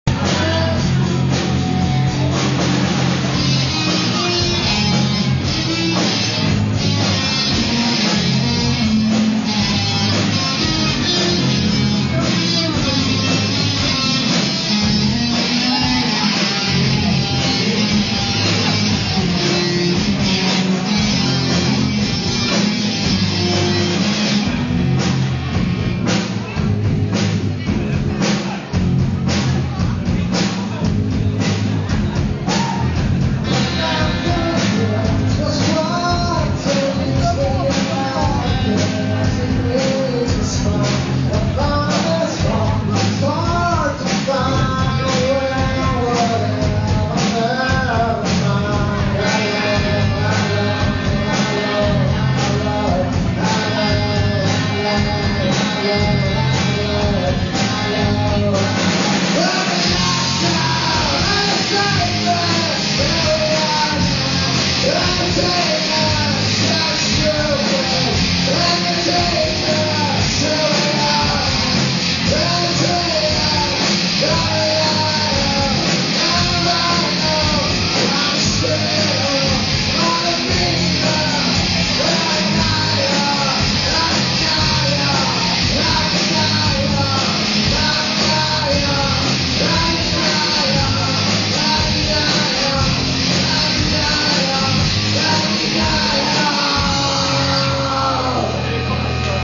spelning i värnamo :p
Ingen aning om vilka/vem som sjunger, men låten har dom iaf förstört